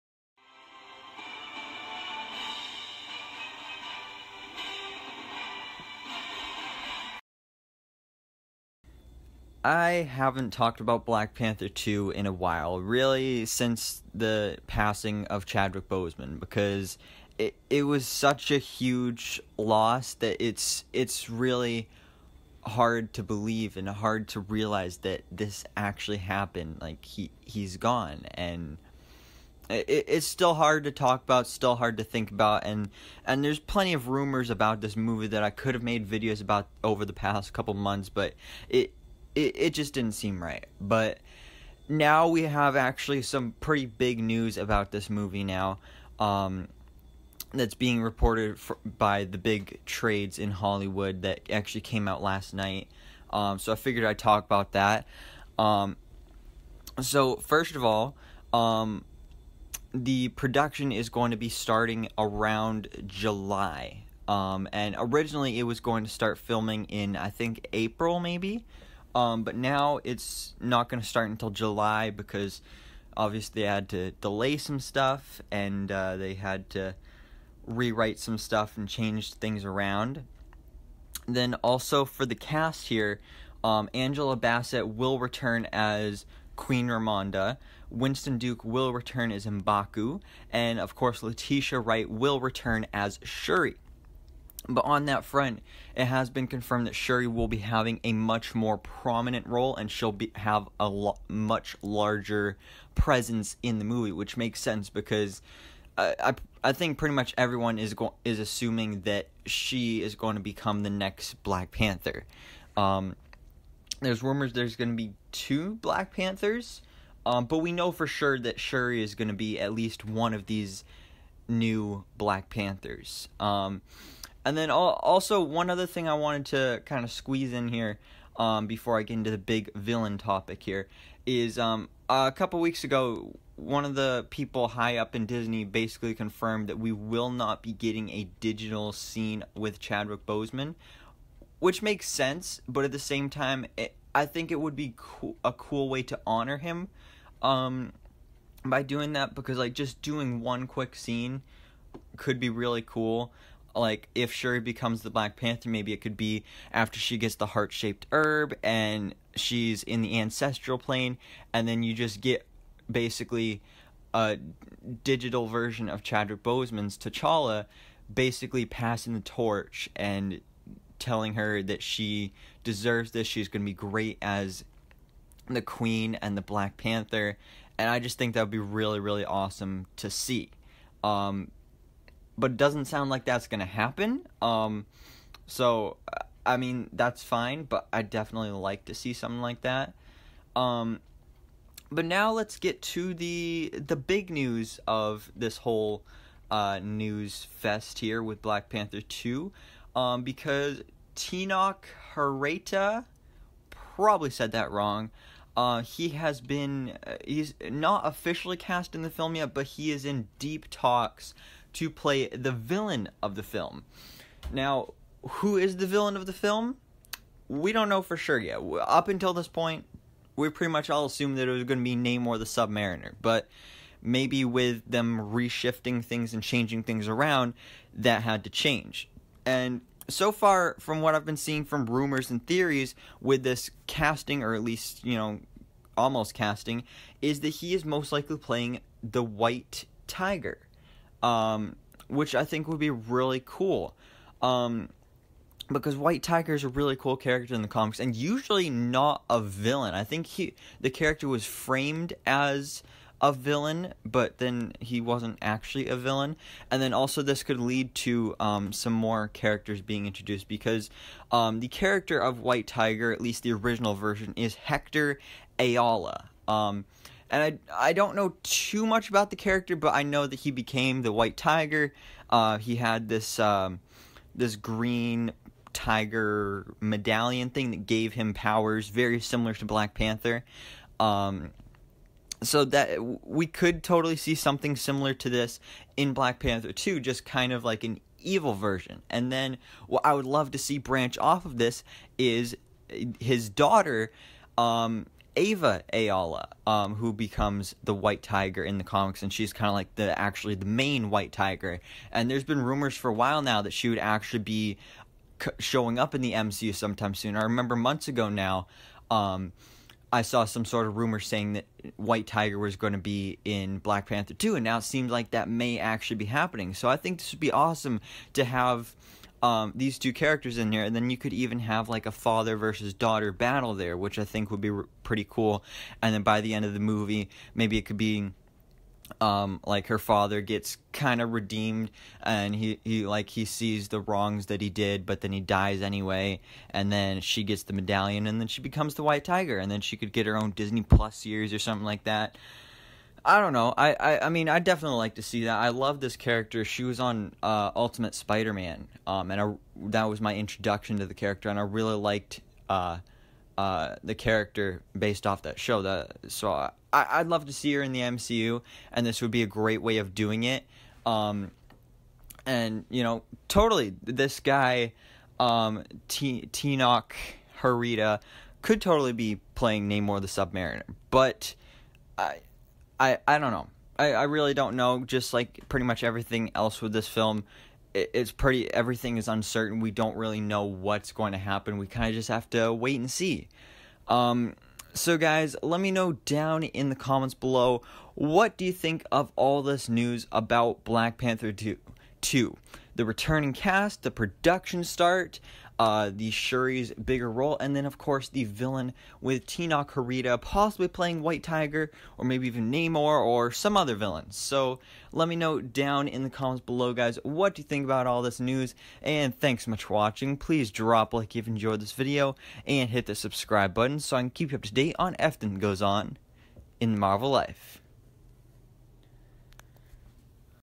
HUGE BLACK PANTHER 2 UPDATE! White Tiger is the Villain! Shuri Will Be Black Panther! Returning Cast
Some new Black Panther 2 details have been revealed including the villain possibly being White Tiger, Shuri becoming Black Panther, production start date, returning cast members, and more.